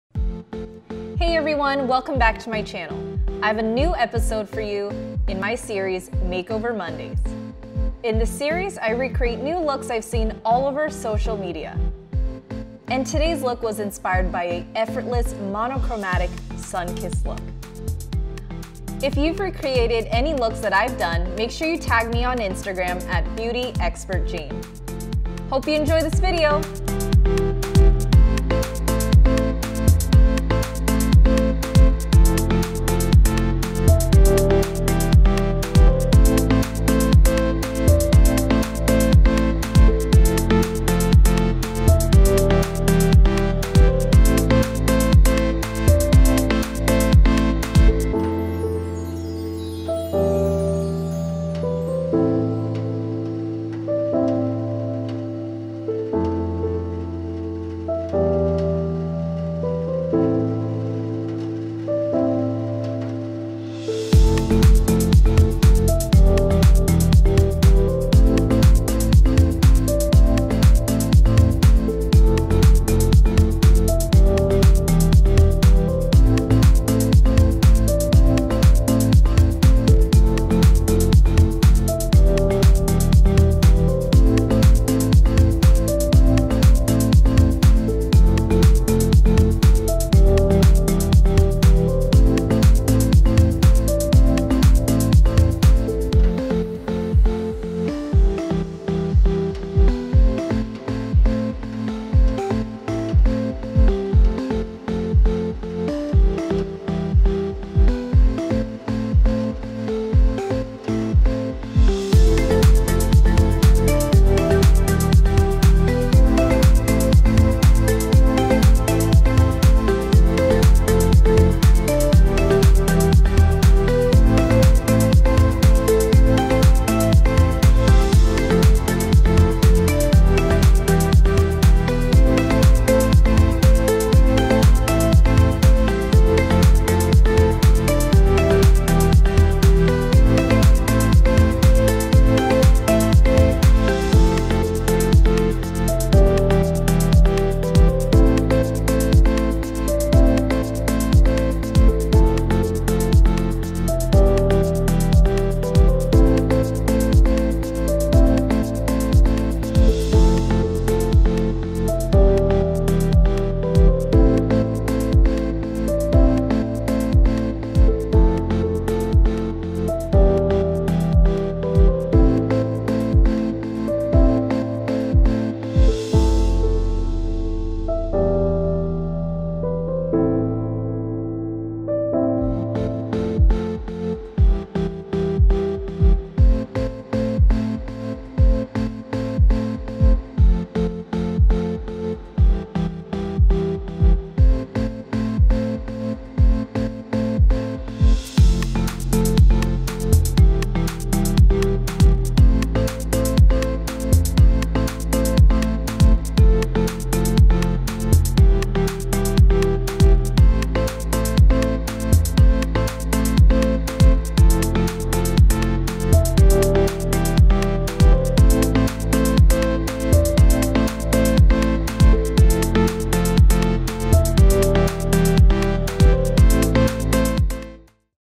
SIMPLE SUMMER NATURAL MAKEUP LOOK- EVERYDAY - FOR BEGINNERS - Makeover Mondays Episode: 7
Hi Everyone!

This is another episode of my series: MAKEOVER MONDAYS!
In this series, I recreate beautiful looks I've seen all over social media.

In this video, I am recreating a summer soft glam makeup look.
This effortless look is great for every occasion!

If you get a chance to recreate this look, tag me on Instagram:
and I'd love to re-post your creation!

Products Mentioned:
NARS Narsissist Loaded Eyeshadow Palette
NARS Wanted Blush Palette
ESTEE LAUDER Futurist Hydra Rescue Moisturizing Makeup in 3W1
Maybelline Fit Me Foundation in Coconut
MAC 24 hour liquid concealer in NC 35
ESTEE LAUDER Bronze Goddess Highlighter Powder in 01 Heat Wave
JULEP Length Matters Mascara
MAC Lipglass in Goldfinsh
MAC Eye Pencil in Teddy